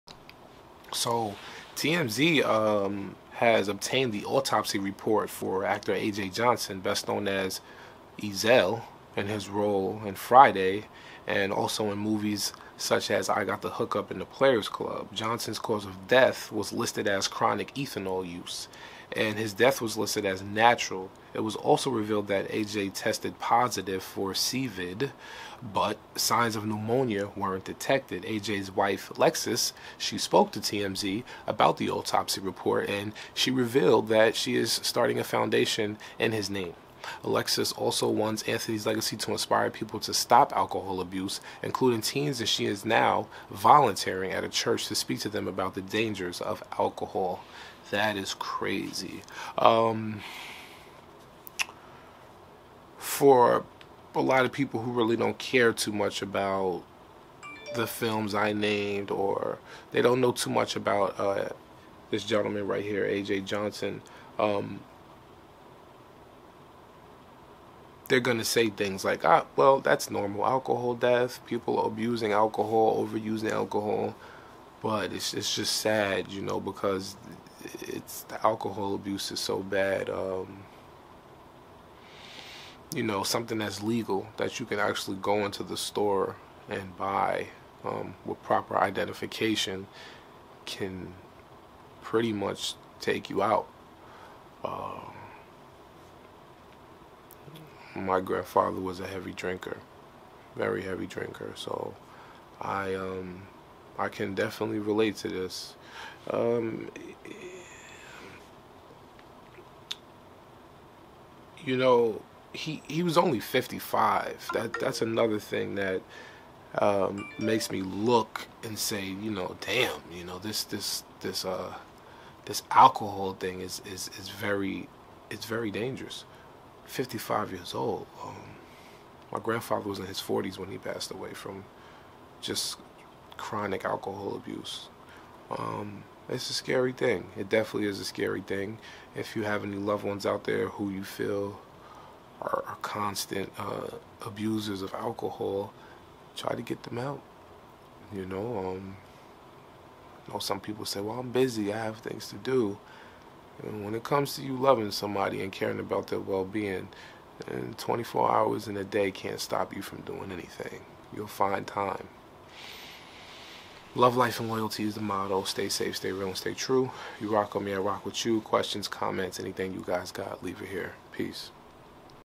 AJ JOHNSON AUTOPSY REPORT
TMZ obtained the autopsy report for actor AJ Johnson, best known for his roles in Friday, I Got the Hook-Up, and The Player's Club.
Johnson's cause of death was listed as "chronic ethanol use," and his death was listed as natural. It was also revealed that AJ tested positive for COVID, but signs of pneumonia weren't detected.
AJ's wife, Lexis, spoke to TMZ about the autopsy report, and she revealed that she is starting a foundation in his name. Lexis also wants Anthony's legacy to inspire people to stop alcohol abuse, including teens, and she is now volunteering at a church to speak to them about the dangers of alcohol.
Source: TMZ